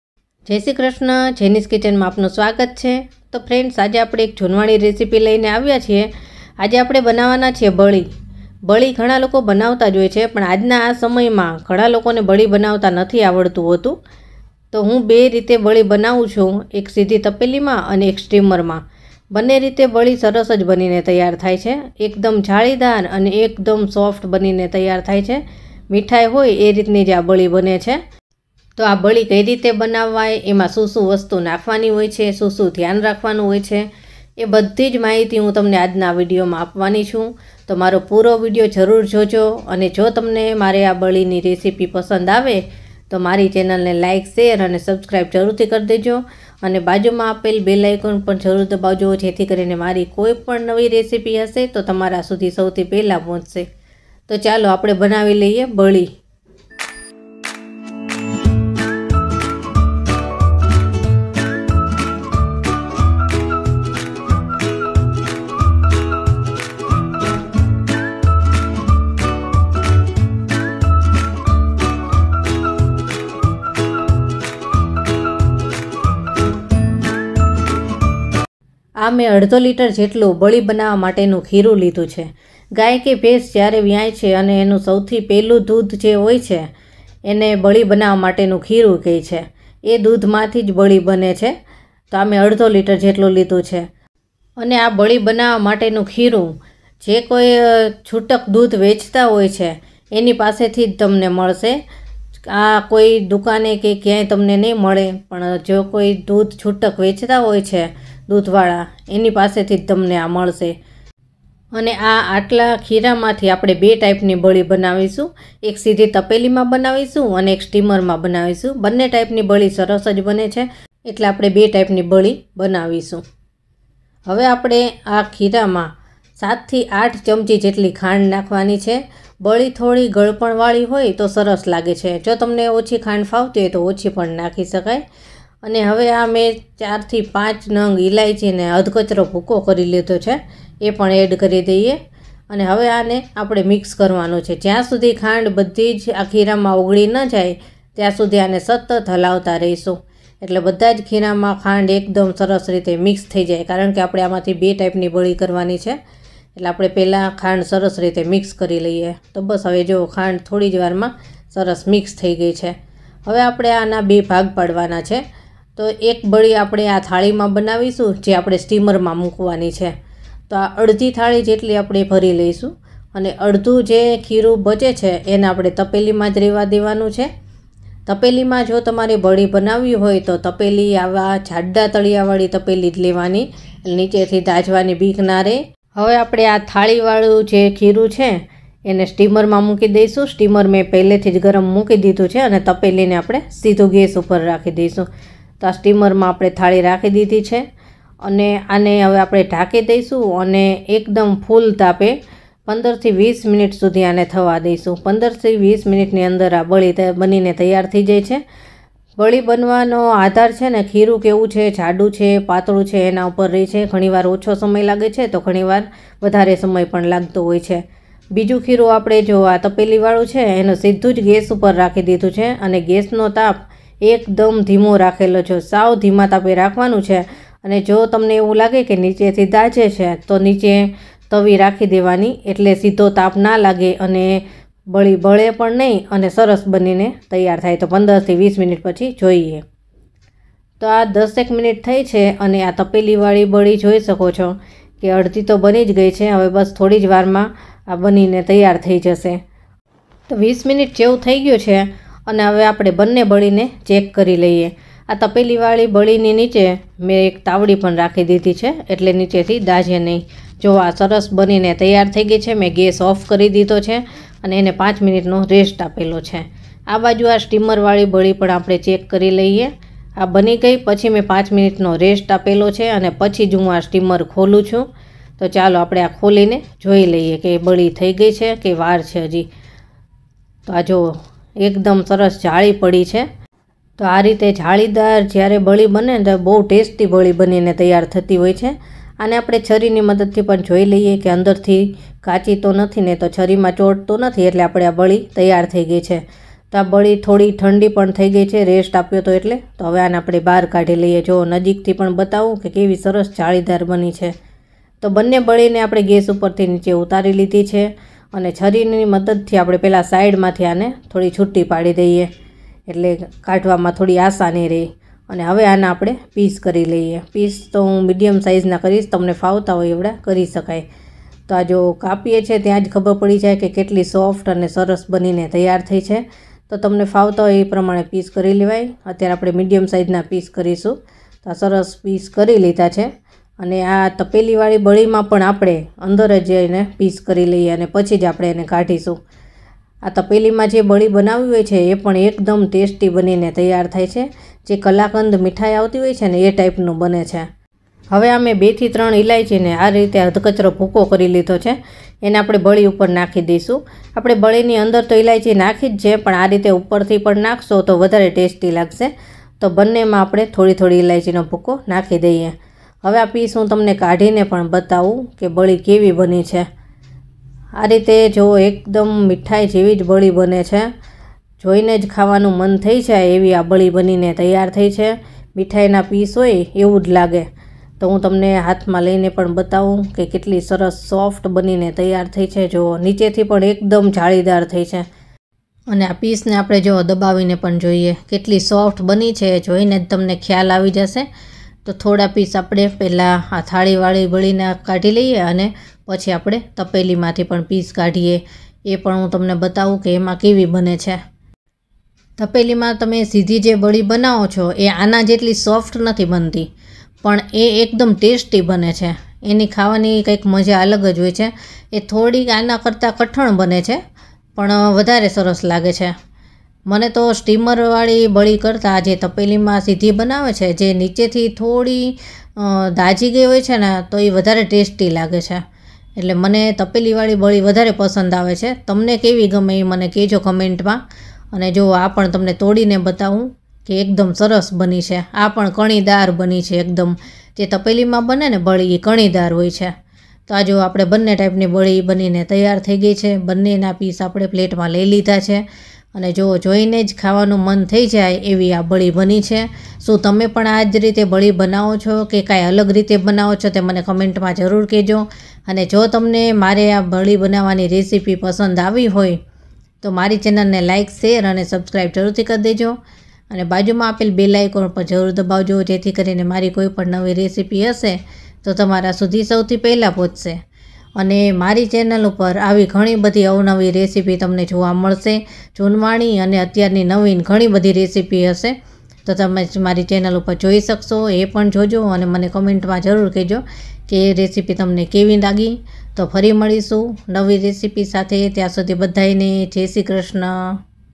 આ રીતે બળી બનાવશો તો એકદમ મીઠાઈ જેવીજ બનશે|Bali Recipe| kharvas Recipe
hello friends how are you hope you are doing well friends aaj hum banane vale hai Bali (kharvas) Recipe vo bhi do tarike se aur bilkul mithai jaise hi banke taiyar hoti hai agar aapko bhi ye recipe sikhni hai to meri puri video jarur dekhna aur agar aap ko meri video pasand aaye to meri channel ko like share subscribe aur comment karna mat bhulna thankyou.